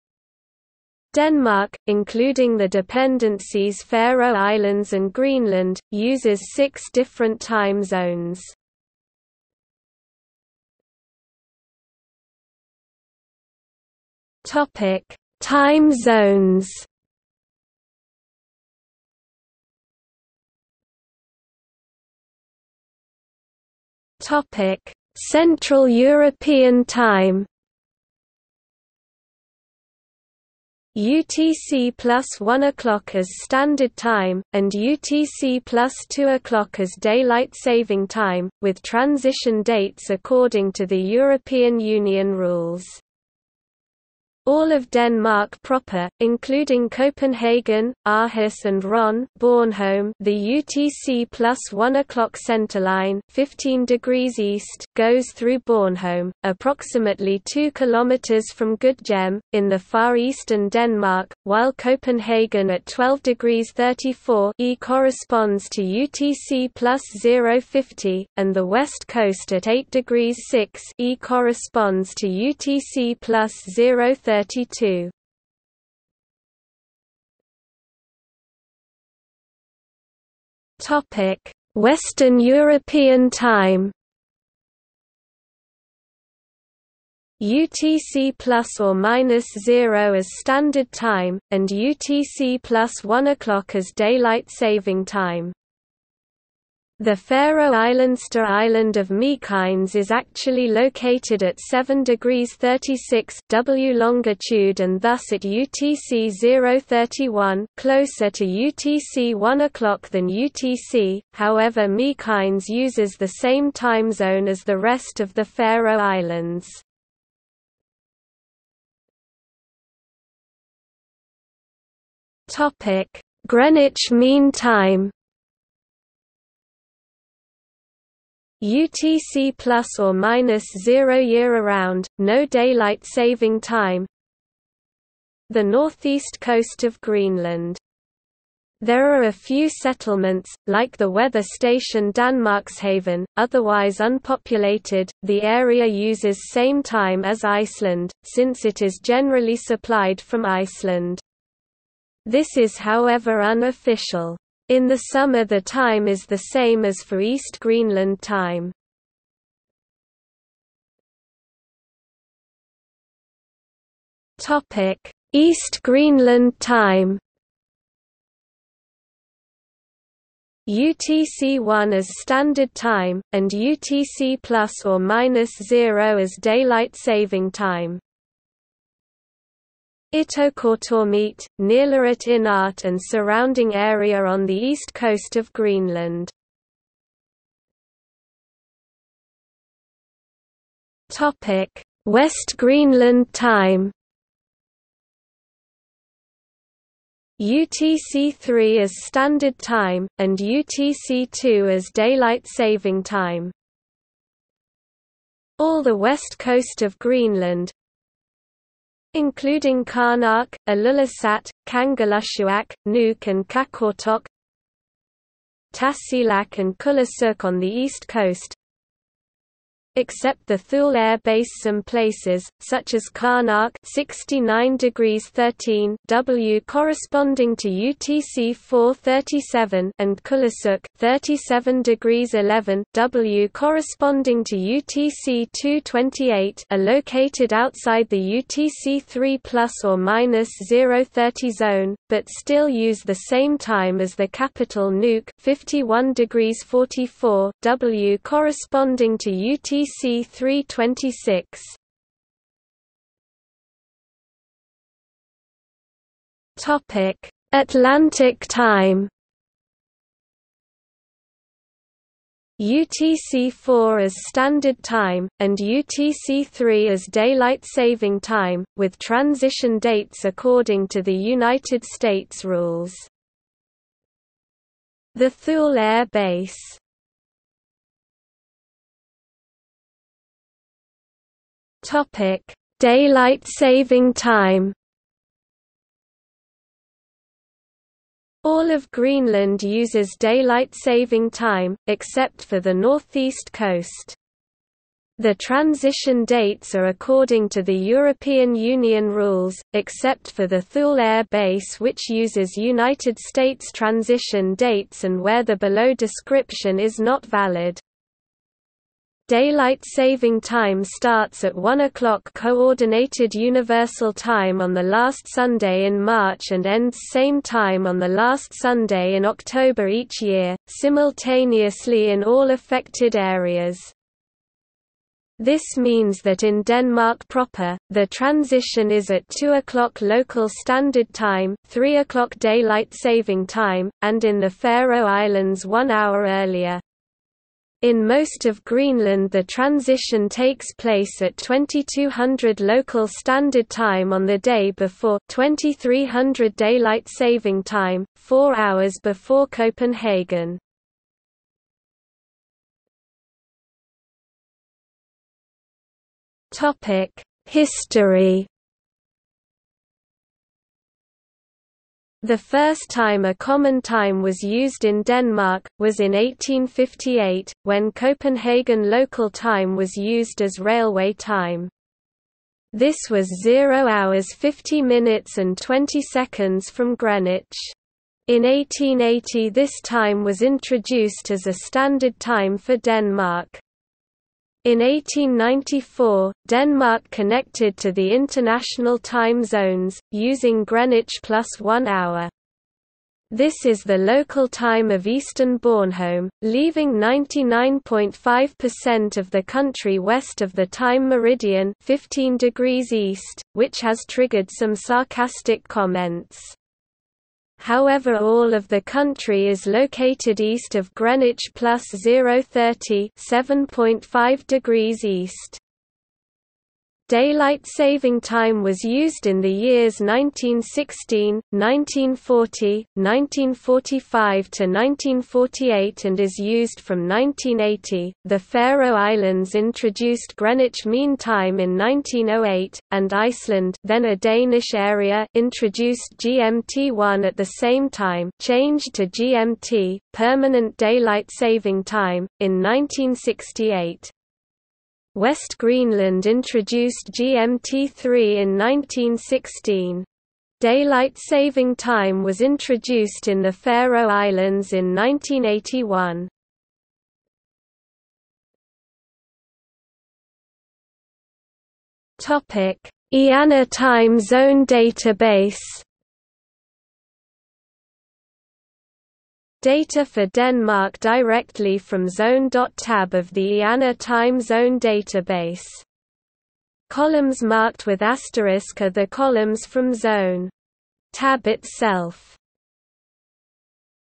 Western Greenland Summer Time | Wikipedia audio article
00:00:10 1 Time zones
00:00:20 1.1 Central European Time
00:01:17 1.2 Western European Time
00:01:59 1.3 Greenwich Mean Time
00:02:41 1.4 East Greenland Time
00:03:07 1.5 West Greenland Time
00:04:13 1.6 Atlantic Time
00:04:38 2 Daylight saving time
00:05:52 3 History
00:07:38 4 IANA time zone database

- Socrates

SUMMARY
Denmark, including the dependencies Faroe Islands and Greenland, uses six different time zones.